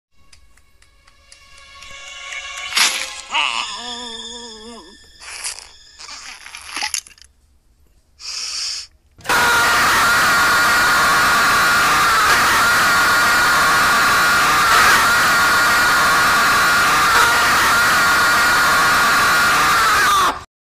Cricket Steps on a Lego Brick but with Tyler1 Scream
Ouch! That's gotta hurt! And it sure is really loud.